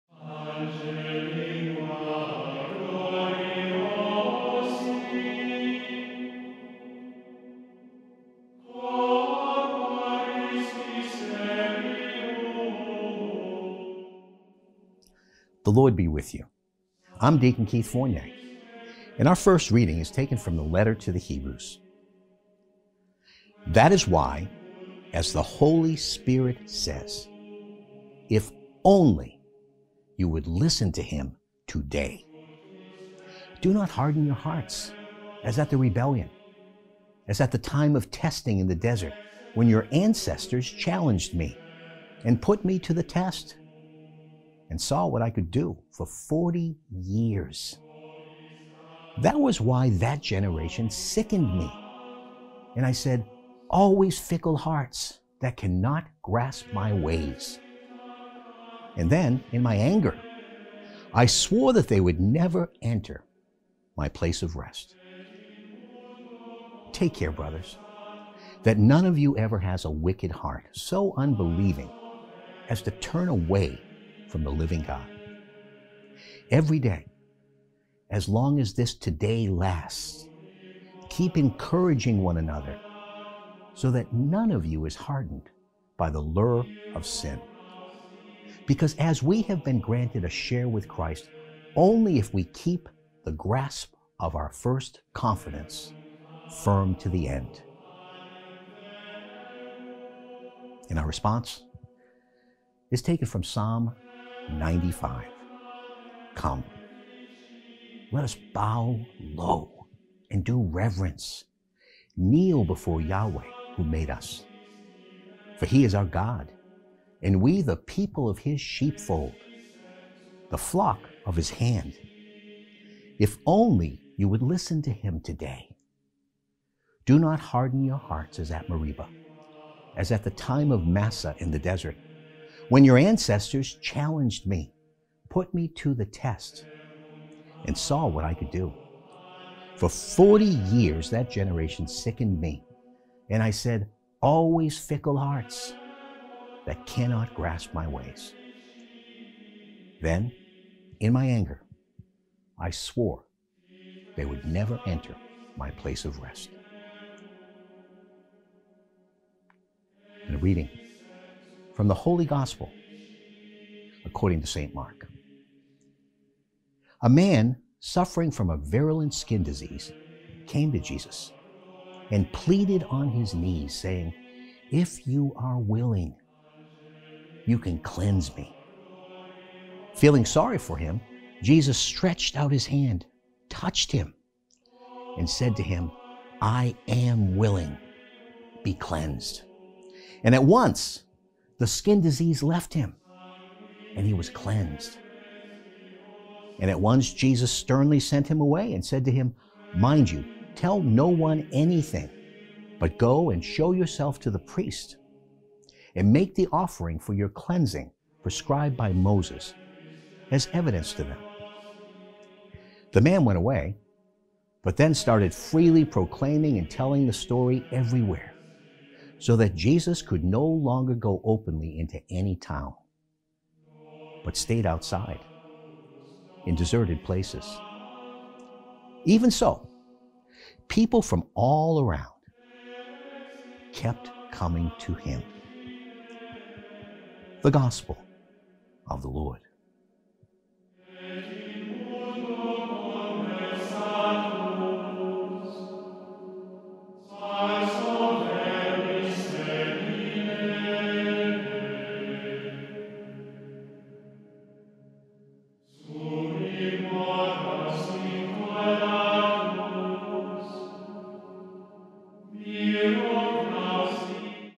Daily Reading for Thursday, January 14th, 2021 HD
Reading 1, Hebrews 3:7-14
7 That is why, as the Holy Spirit says: If only you would listen to him today!

8 Do not harden your hearts, as at the rebellion, as at the time of testing in the desert,

9 when your ancestors challenged me, and put me to the test, and saw what I could do

10 for forty years. That was why that generation sickened me and I said, 'Always fickle hearts, that cannot grasp my ways!'

11 And then in my anger I swore that they would never enter my place of rest.

12 Take care, brothers, that none of you ever has a wicked heart, so unbelieving as to turn away from the living God.

13 Every day, as long as this today lasts, keep encouraging one another so that none of you is hardened by the lure of sin,

14 because we have been granted a share with Christ only if we keep the grasp of our first confidence firm to the end.

6 Come, let us bow low and do reverence; kneel before Yahweh who made us!

7 For he is our God, and we the people of his sheepfold, the flock of his hand. If only you would listen to him today!

8 Do not harden your hearts as at Meribah, as at the time of Massah in the desert,

9 when your ancestors challenged me, put me to the test, and saw what I could do!

10 For forty years that generation sickened me, and I said, 'Always fickle hearts; they cannot grasp my ways.'

11 Then in my anger I swore they would never enter my place of rest.

40 A man suffering from a virulent skin-disease came to him and pleaded on his knees saying, 'If you are willing, you can cleanse me.'

41 Feeling sorry for him, Jesus stretched out his hand, touched him and said to him, 'I am willing. Be cleansed.'

42 And at once the skin-disease left him and he was cleansed.

43 And at once Jesus sternly sent him away and said to him,

44 'Mind you tell no one anything, but go and show yourself to the priest, and make the offering for your cleansing prescribed by Moses as evidence to them.'

45 The man went away, but then started freely proclaiming and telling the story everywhere, so that Jesus could no longer go openly into any town, but stayed outside in deserted places. Even so, people from all around kept coming to him.